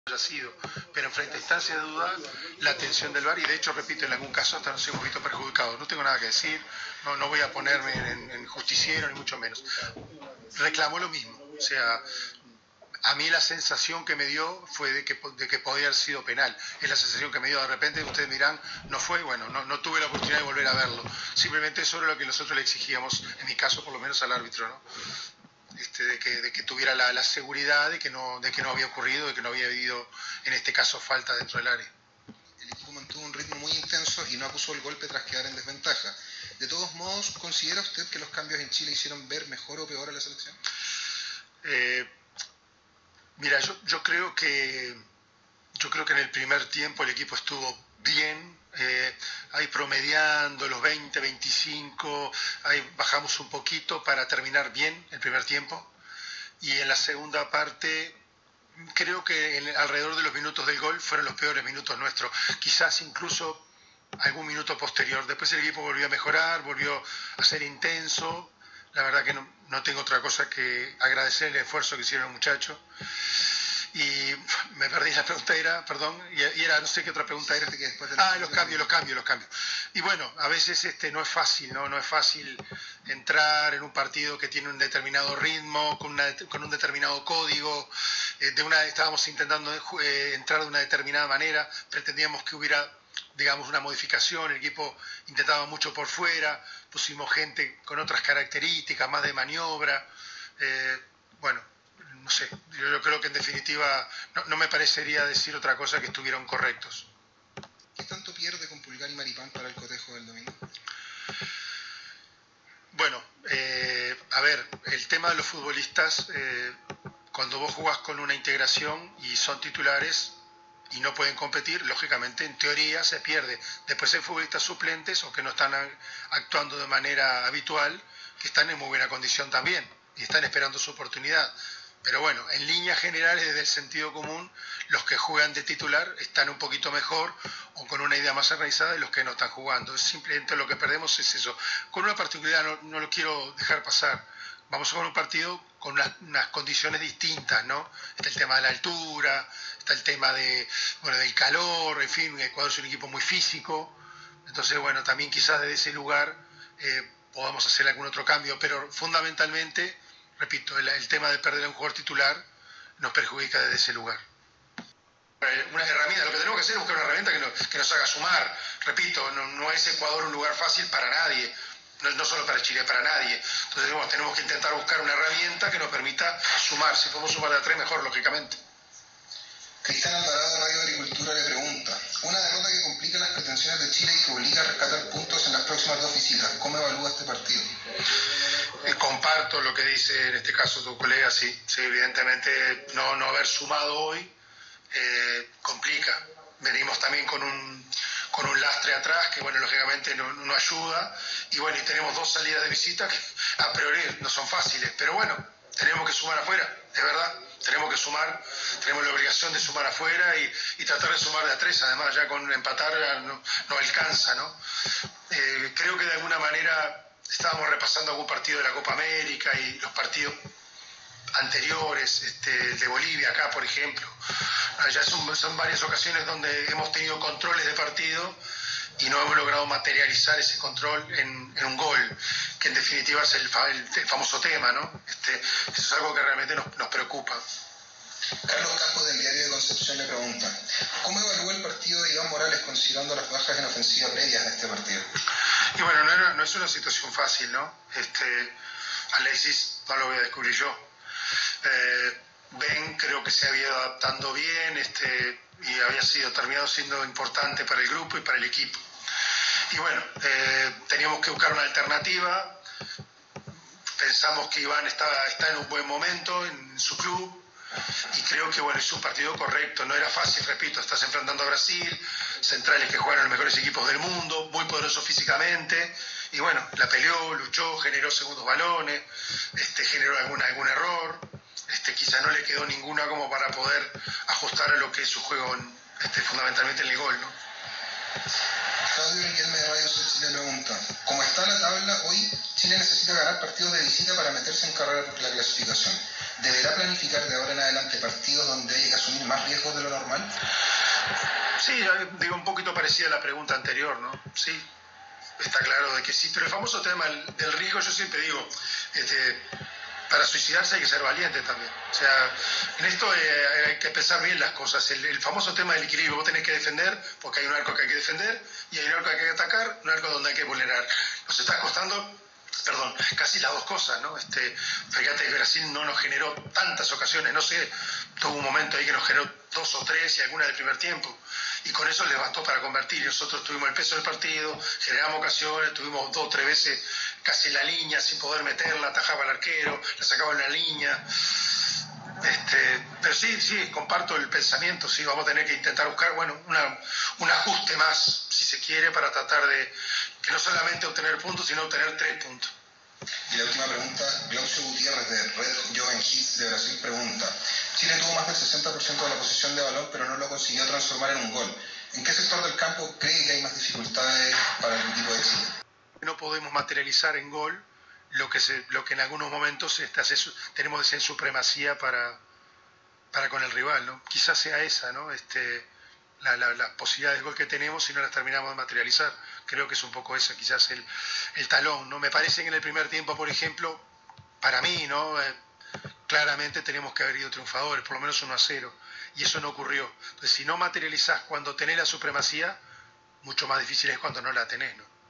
Eliminatorias: Chile 0 - 1 Brasil - Conferencia de prensa de Martín Lasarte
Brasil ganó en el Estadio Monumental.